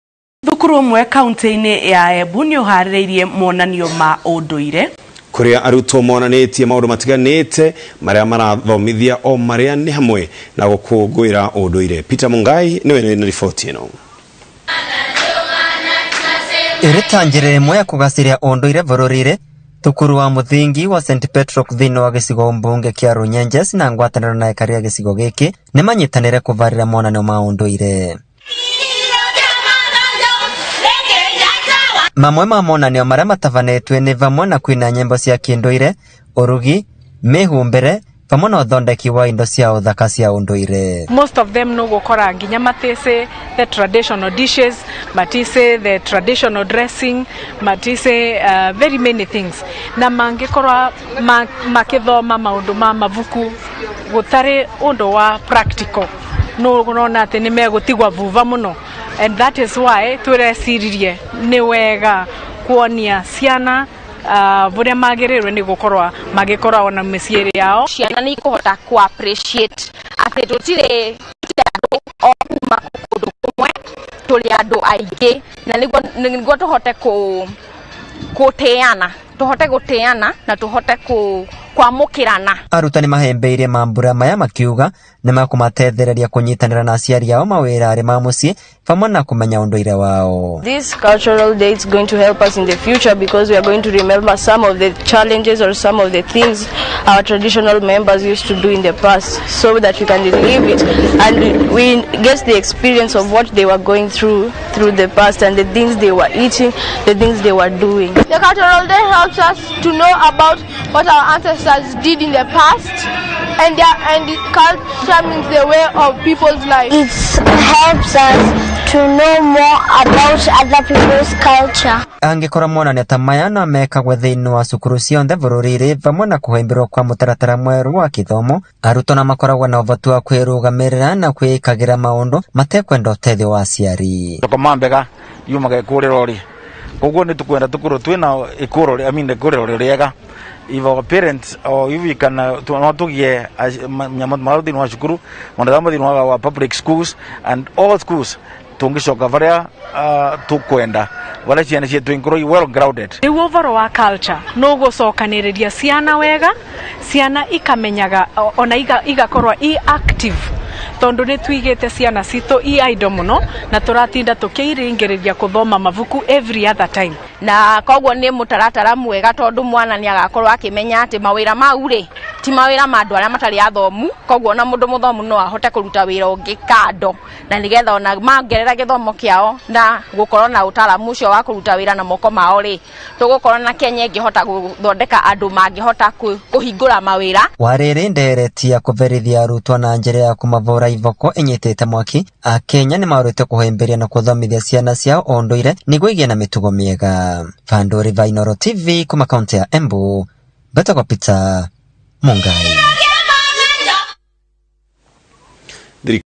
Inooro TV
St. Petroc Premier School is a day and boarding CBC & 844. We are the HomeofExcellence.

See the Cultural Day Fair held on the 10th of December 2021. Thank you always for your support and prayers. Remember to check more details on our

To our pupils- Impressive
To our teachers- Formidable
To our parents- Stunning
To our directors- Remarkable
To our guest- Awe-Inspiring